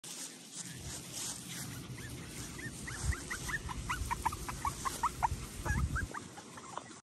August 21, 2023
Poodle puppies at play.  They are AKC Toy puppies and 6 weeks of age in video.